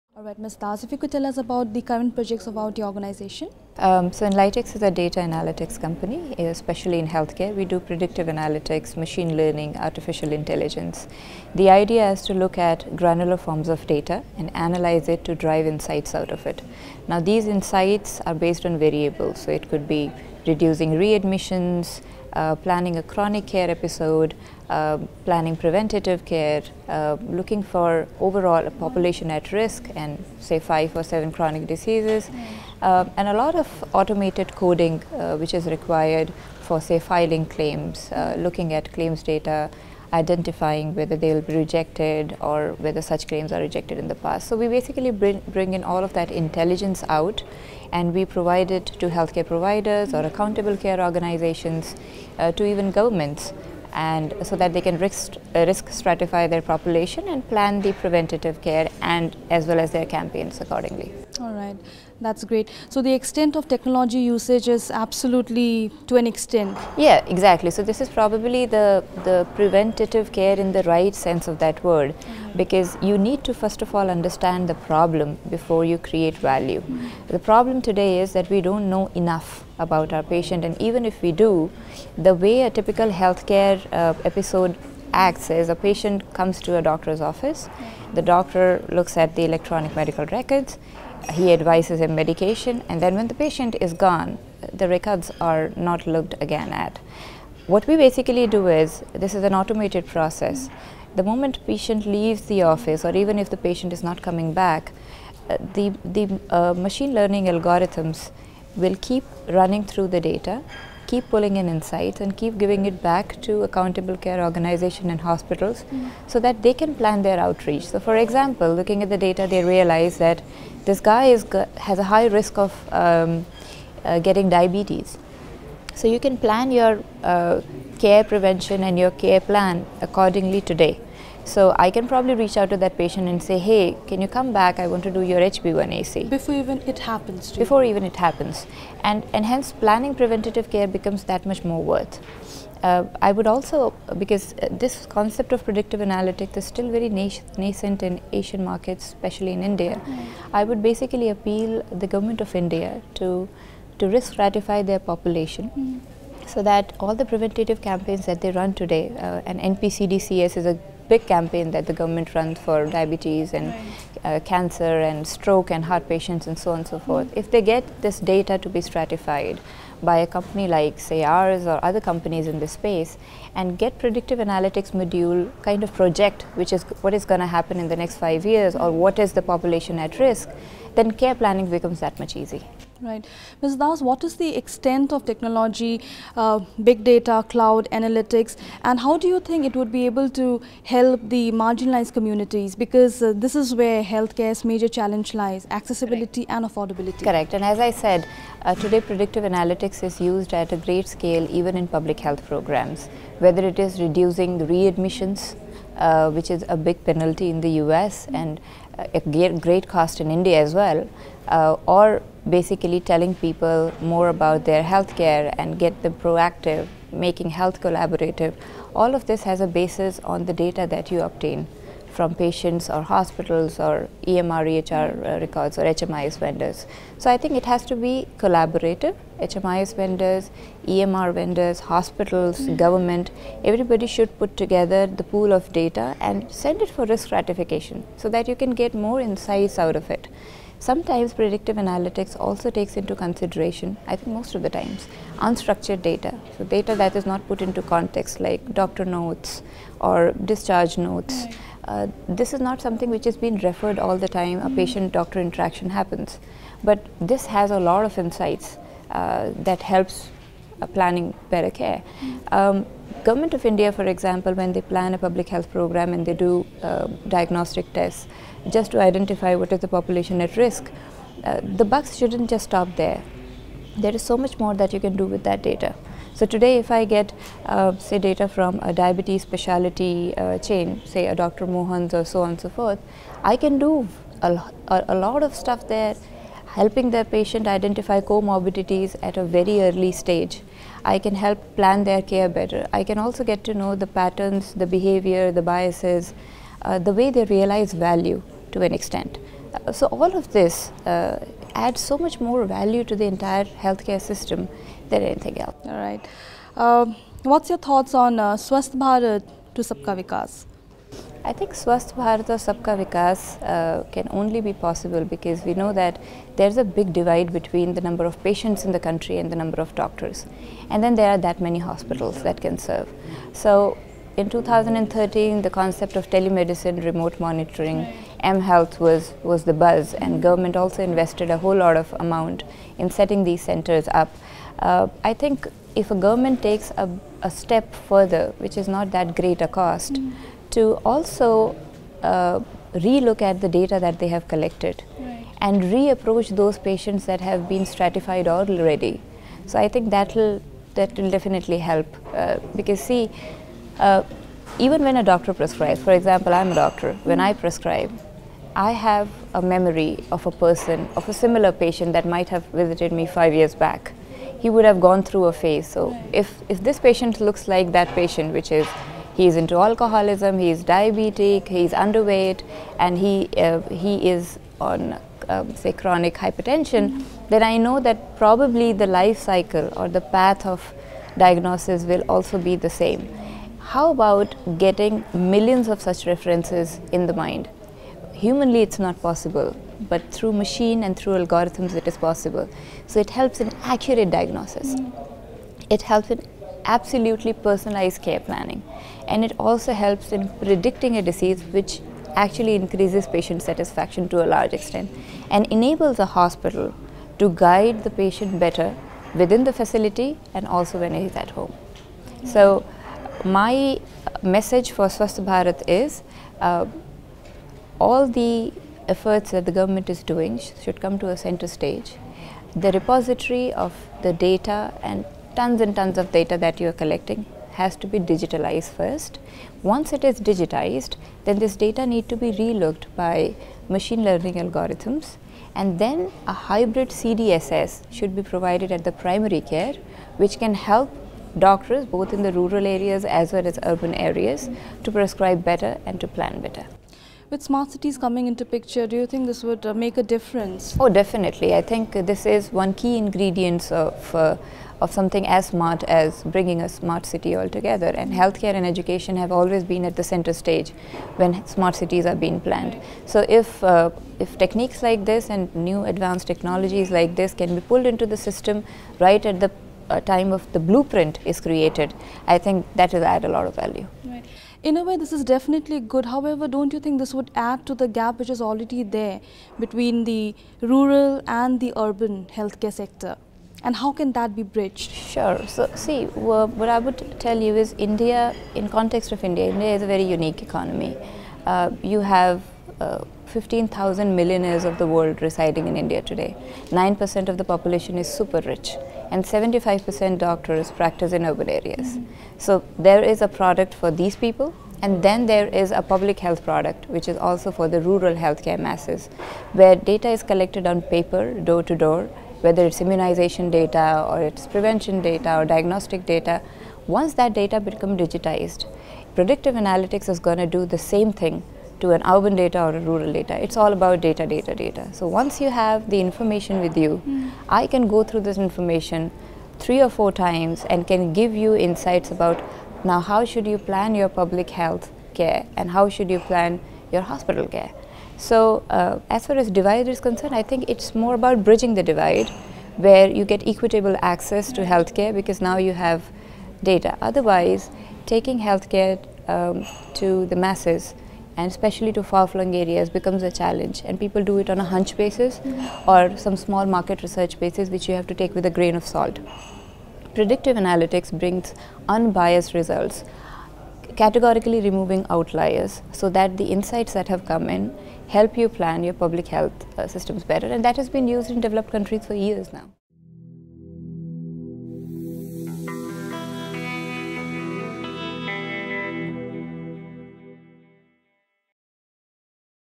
The Problem Today Is: We Don't Know Enough About Our Patients', Says Dr. Ruchi Dass of Enlightiks
Dr. Ruchi Dass, CMO & Head Clinical Analytics, Enlightiks Inc.(USA) spoke to BW|Businessworld's Sapna Bhardwaj on the sidelines of 2nd Healthcare Summit, recently held on 5th June 2015. We spoke about use of advanced analytics and Big Data in Healthcare. Enlightiks is a data analytics company based out of Palo Alto and has a data science lab in Bangalore, India. Dr Dass is considered as a thought leader in the space of predictive analytics, machine learning and artificial intelligence. Enlightiks platform "Querent" looks at granular form of data and analyses it to drive insights. We discussed how can the major challenge of affordability and accessibility in healthcare be addressed through data analytics and what is the potential and possible use cases of such technologies.
Credits : Editing - Vijay Shankar, Camera - Ratnesh Singh, Head Video Editorial - Sapna Bhardwaj